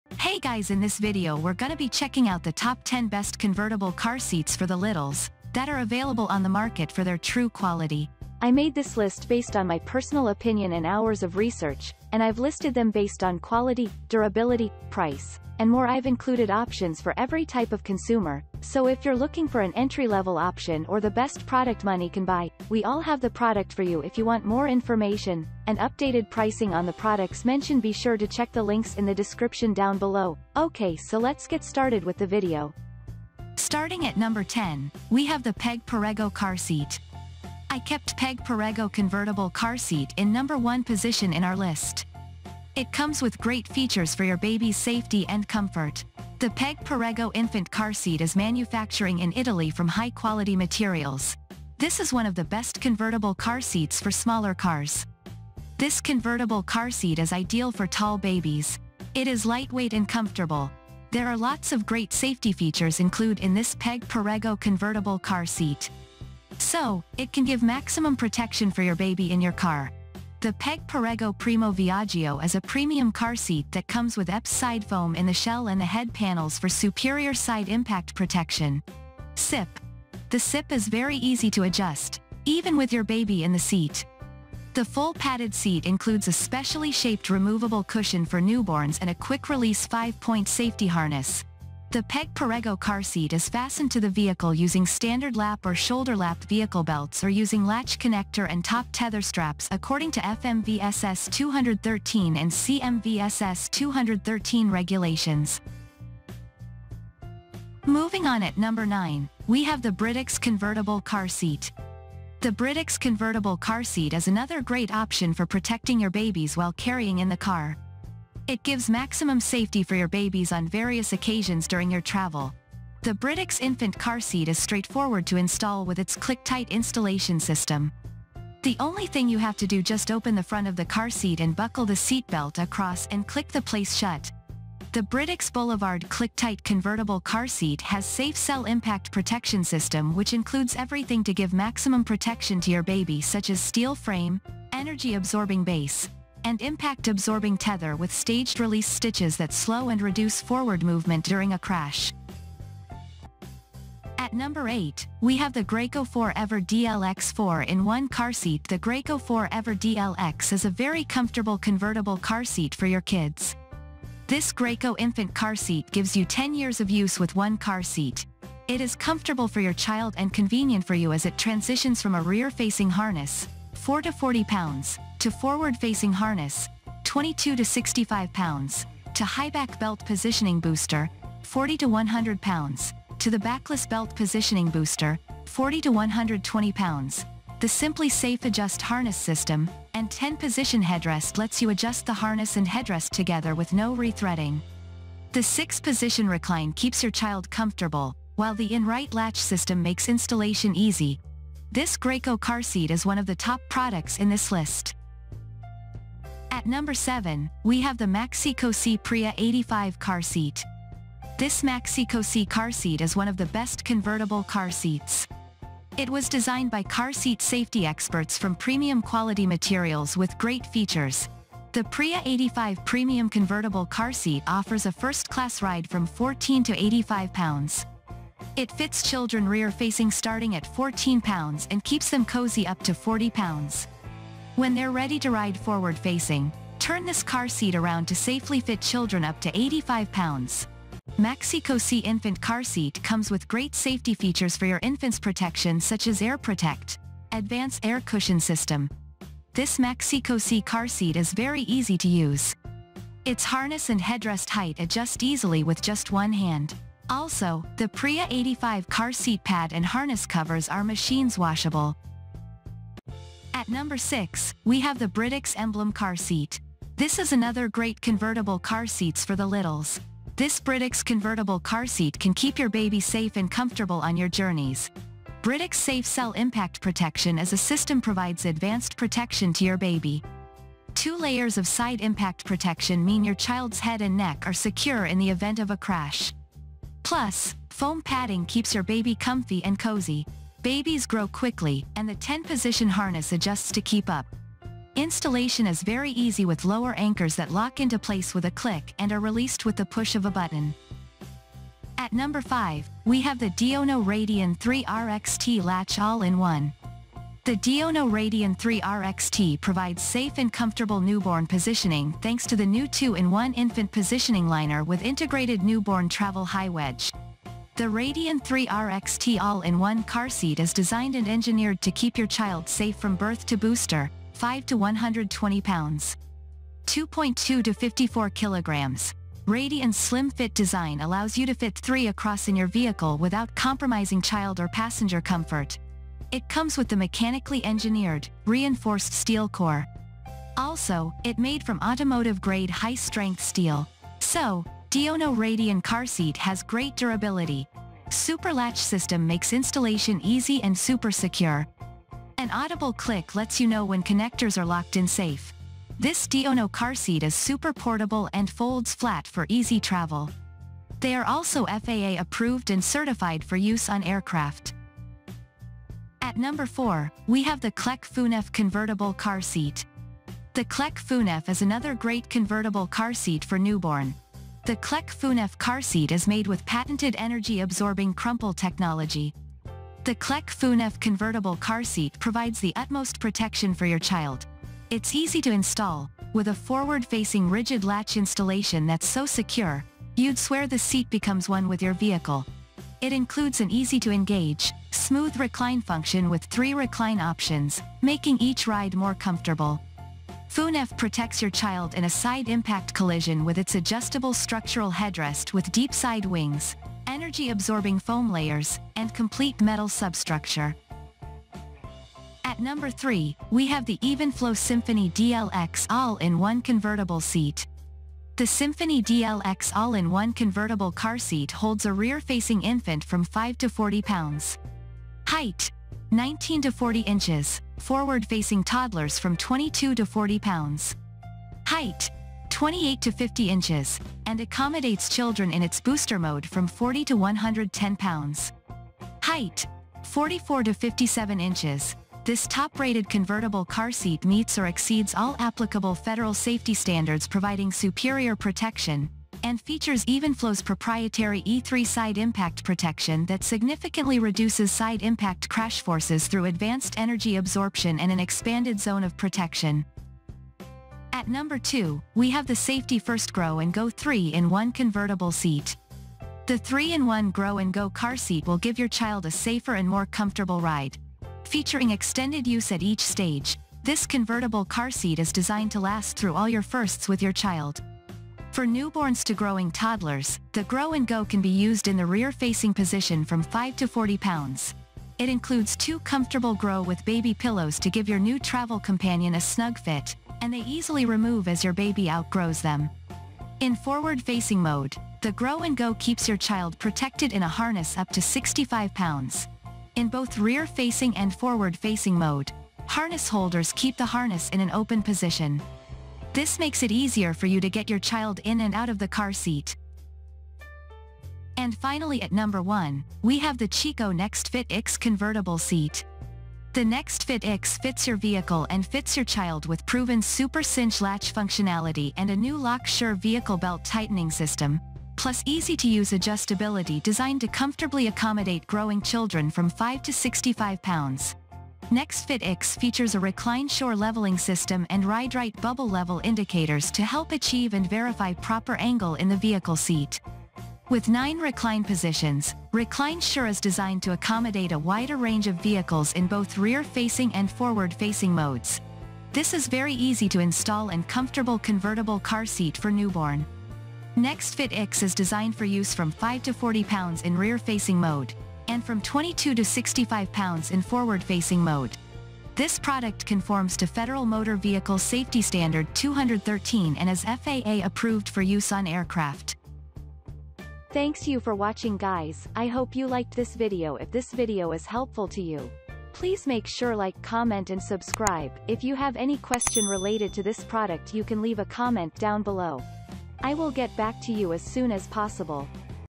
✅Top 10 Best Convertible Car Seats For The Littles 2024
➜ Links to the Best Convertible Car Seats  we listed in this video:
Check Price on aliexpress
🔶 10.Peg Perego Car Seat
🔶 9.Britax Convertible Car Seat
🔶 8.Graco 4Ever DLX 4 in 1 Car Seat
🔶 7.Maxi-Cosi Pria 85 Car Seat
🔶 6.Britax Emblem Car Seat
🔶 5.Diono Radian 3RXT Latch All-in-One
🔶 4.  Clek Foonf Convertible Car Seat
🔶 3.Evenflo Symphony DLX All-In-One Convertible Seat
🔶 2.Safety 1st Grow and Go 3-in-1 Convertible Seat
🔶 1.Chicco Next Fit IX Convertible Seat